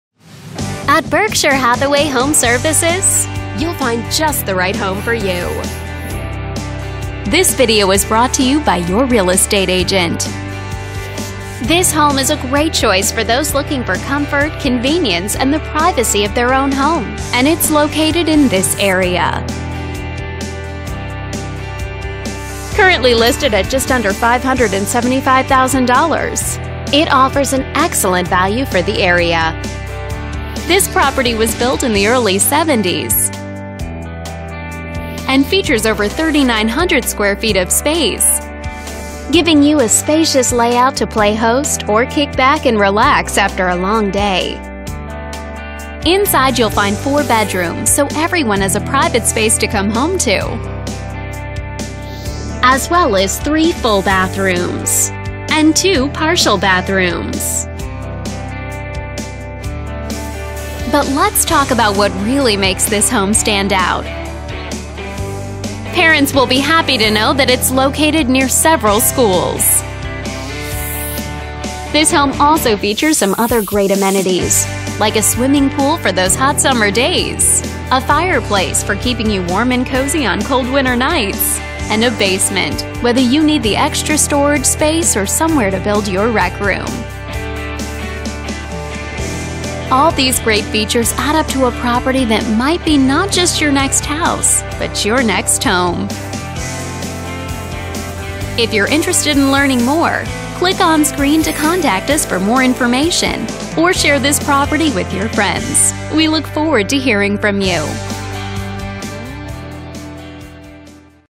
Real Estate for sale in St Louis
MLS#:17031009 Address:10595 Windswept Drive  St Louis 63128 Gorgeous 4 Bedrm 5 Bath 1.5 story with inground pool and 5 car garage in sought after Sunset Hills!!This home offers approx. 4000 sq. ft of open light filled space! Highlights incl: 5+ car garage w/ one slip for Rv/Boat w/ 13 ft door (40x22)!!Wonderful dining room, hearth room, Den and Great Room with coffered ceilings & crown molding!Skylights in Great rm & bkfst room w/ custom plantation shutters.Beautiful kitchen w/bkfst bar, new gas stove, & ceramic flrs! Master br suite on main floor with dual sinks, soaking tub and sep shower! Upstairs has a 2nd master bedroom full bathroom, sitting room, and library, 2 more spacious bedrooms and a full hall bath!The park-like level backyard has patio, in-ground pool, hot tub, gorgeous landscaping, pond with waterfall, and utility shed! Amazing rear entry garage and beautiful Curb appeal! Show & sell!! HSA home warranty free to buyer!Close to Sunset Hill Rec Center, Shopping, Restaurants and Parks!